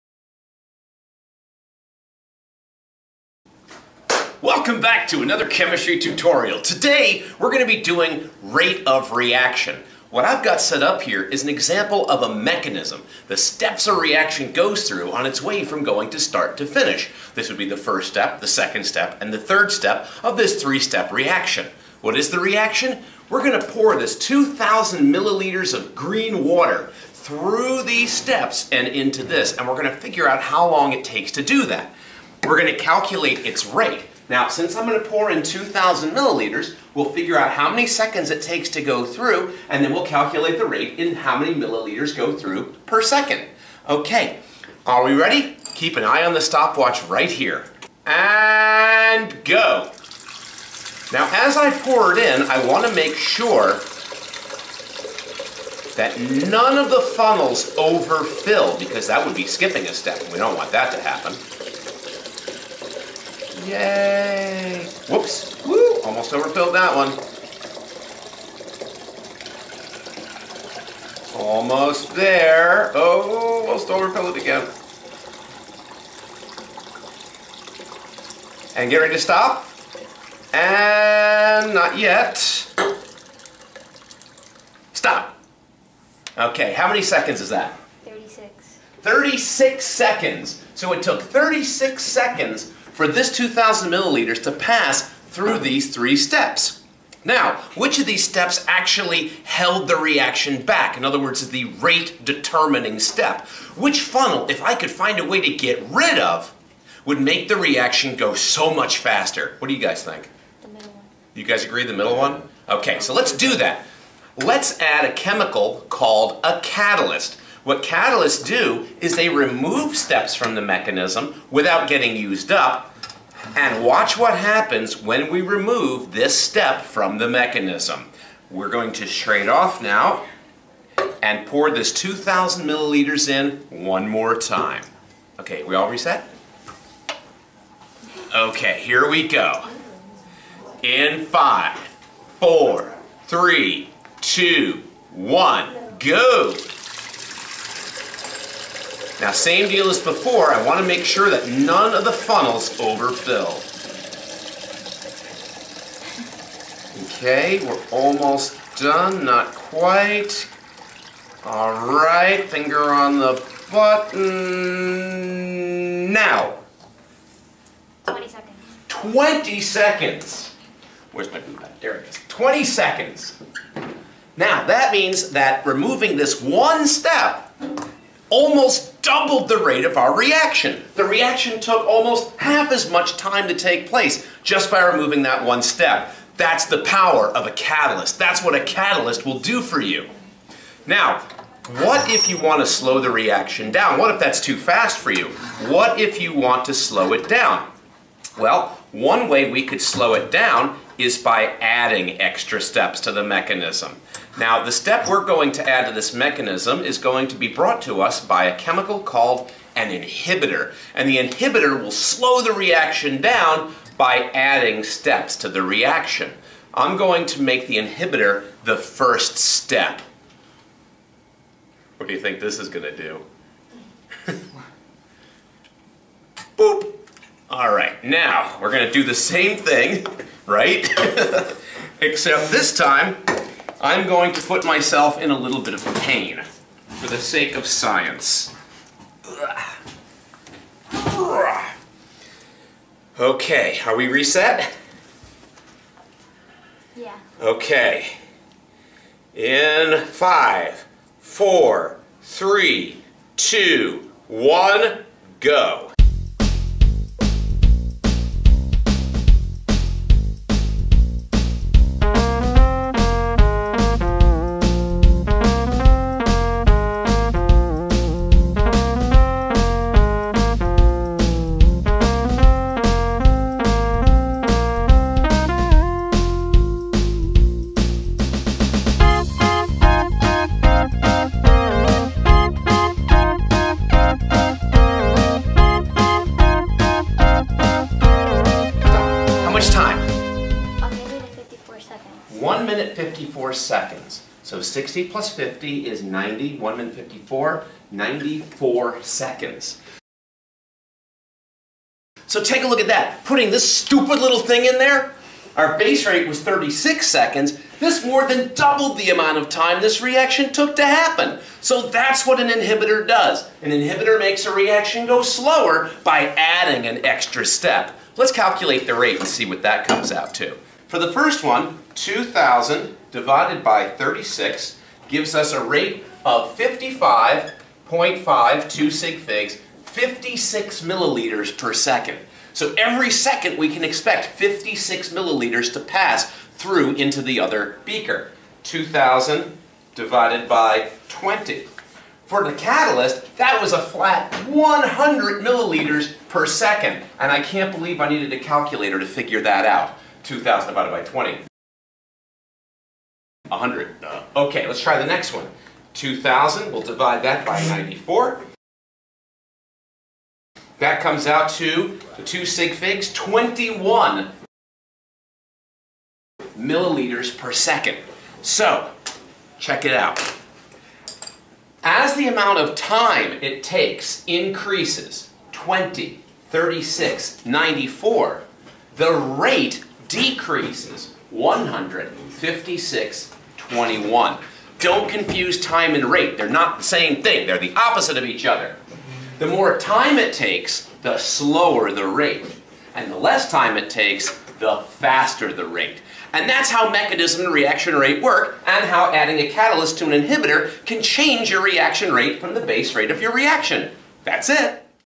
Chem Demo: Reaction Mechanism and Rate
This is a video that depicts what happens when catalysts and inhibitors are added to a reaction.